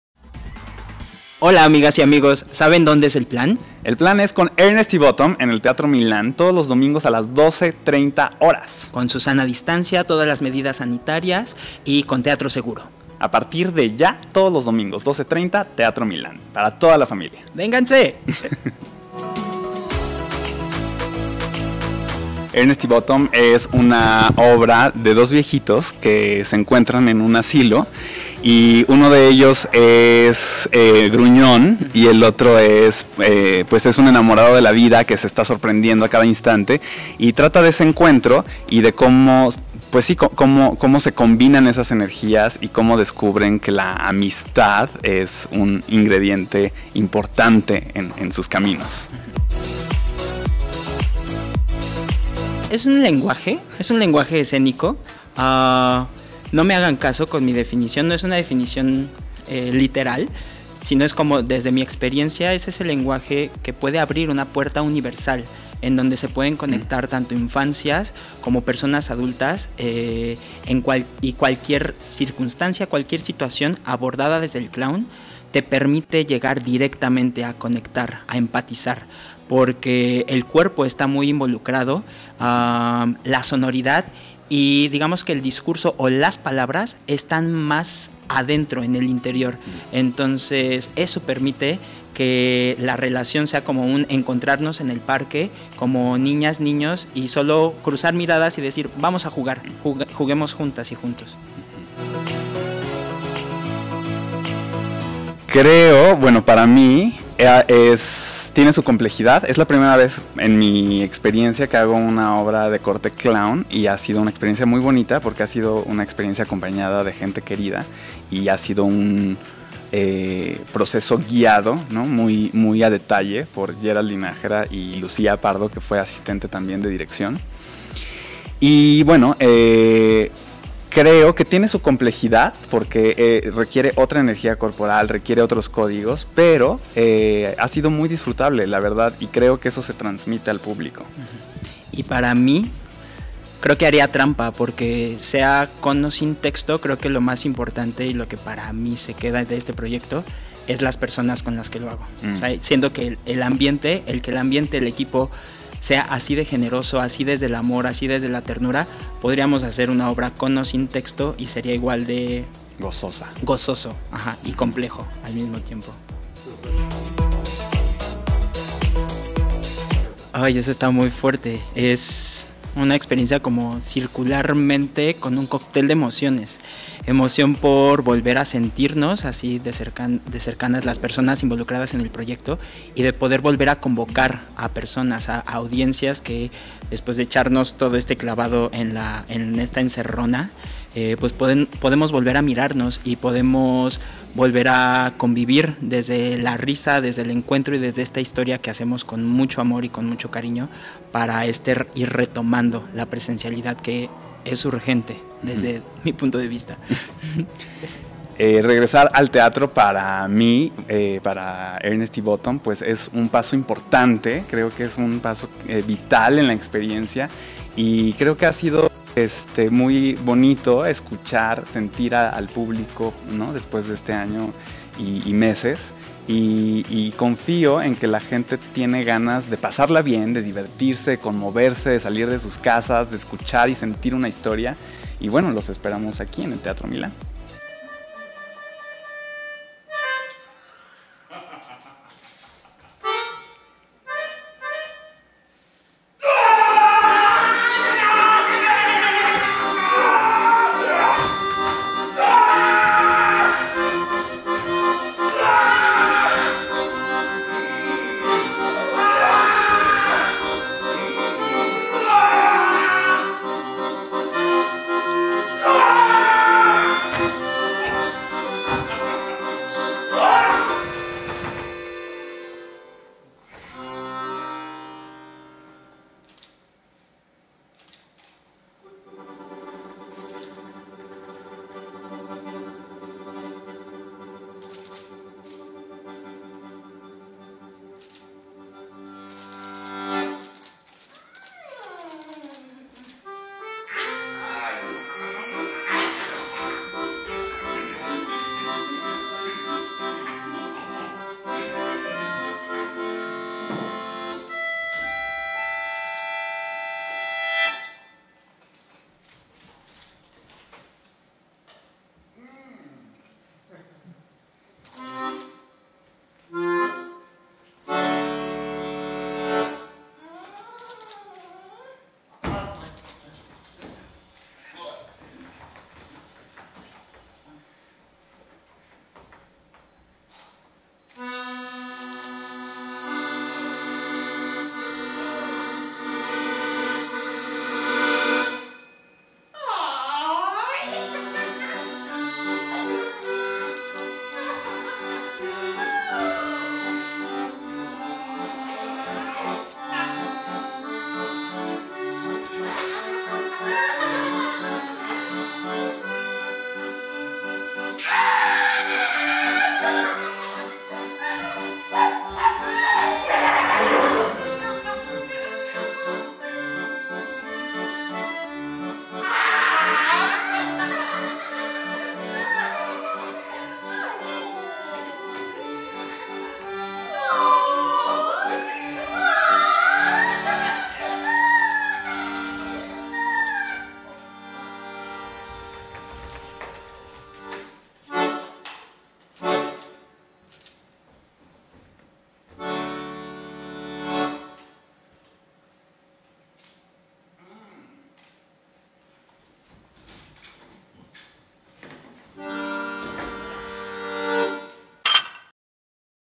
Ernest y Bottom en el Teatro Milán
Nuestro plan fue en ErnestyBottom en el TeatroMilán, platicamos con ellos y nos cuentan de este gran regreso al teatro presencial.

Ernest y Bottom es una obra clown, divertida y conmovedora, para toda la familia, en la que dos viejitos de personalidades opuestas comparten habitación en una casa de descanso. Ernest es serio, ordenado, gruñón y apegado a las cosas materiales; por el otro lado, Bottom es sencillo, alegre y valora las pequeñas cosas de la vida.

En cualquier tiempo y lugar jamás se hubieran relacionado; pero ahora, las circunstancias los llevarán a hacer equipo y embarcarse en un viaje extraordinario hacia una amistad entrañable, recordándonos que la felicidad y la vida es más dulce y divertida si se comparte con un amigo.

Esta divertida y conmovedora obra clown para toda la familia nos llevará a una aventura extraordinaria para volver a sonreír y recordar la magia de la vida.

Se presenta todos los domingos a las 12:30 hrs en el TeatroMilán

¡Checa todo lo que nos contaron!